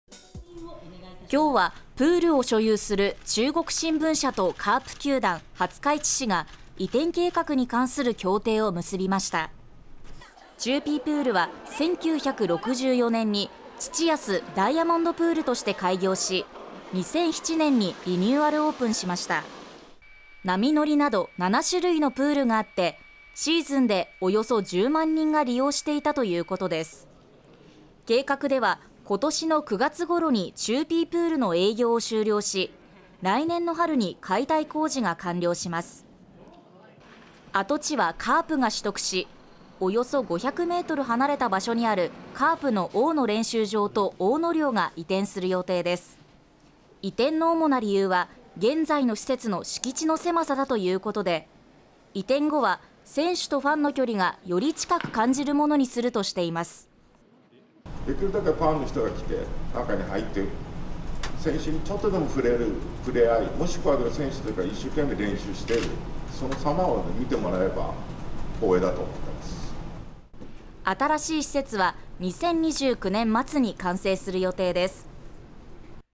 「ちゅーピープール」今夏で営業終了 解体後の跡地にカープ大野練習場＆大野寮が移転へ 2029年末完成予定
廿日市市にある「ちゅーピープール」がこの夏で営業を終え、解体後の跡地にはカープの大野練習場と大野寮が移転することになりました。